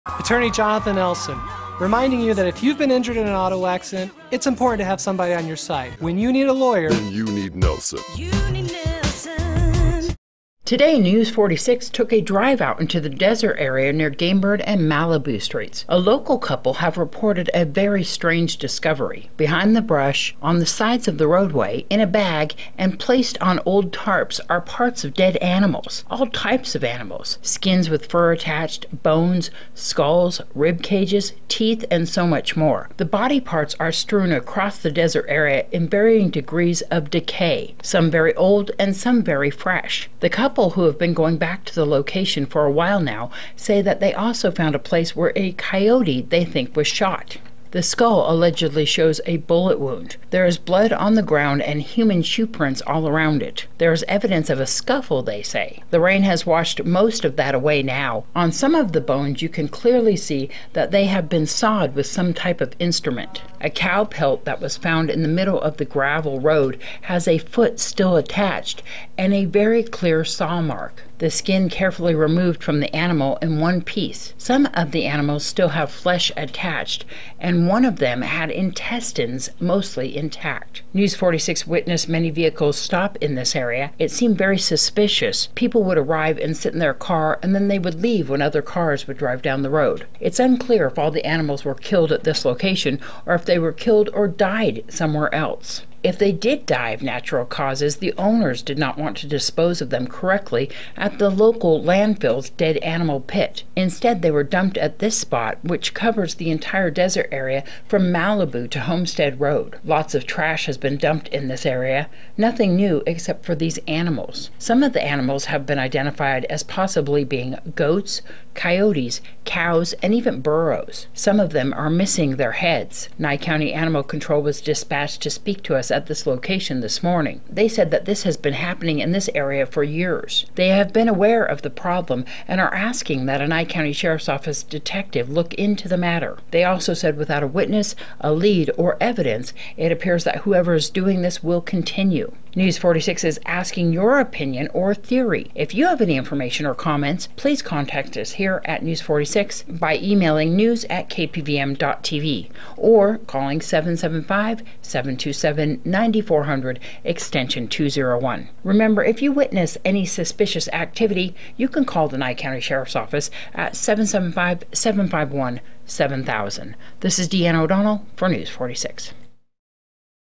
01/12/2018 Dead Animals Found in the Desert
What is going on in the desert near Gamebird Road? A major dumping spot for garbage also appears to be a spot where someone is dumping dead animals. In most cases the animals are not intact. In fact most of the find is skin with the fur attached. We need to warn you that some of these photos may be upsetting to some viewers.